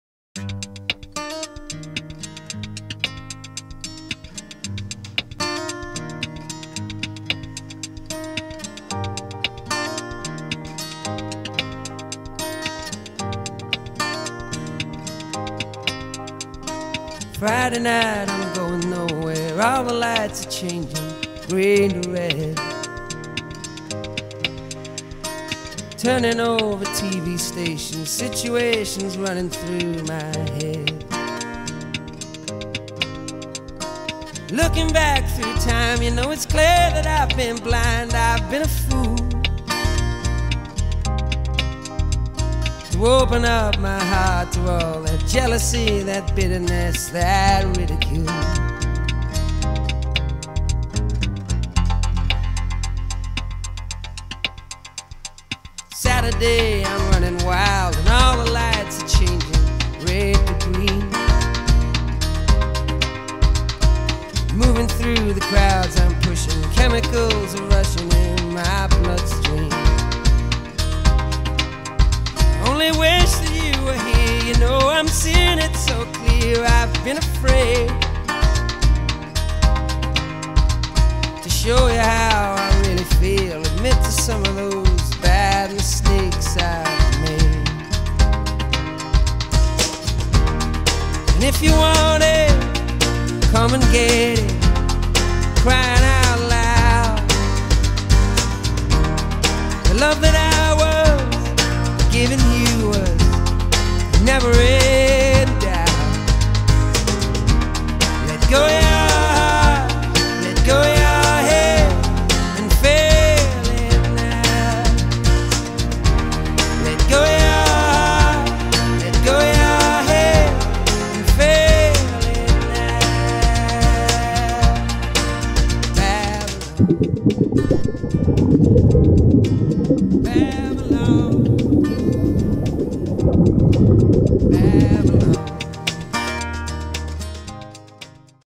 Scuba Diving Mirror Pond Solomon Islands (tunnel cave nature) best diving in the world
This is a dive in the Solomon Islands, Russell islands, @ Mane Island. Its called the Mirror Pond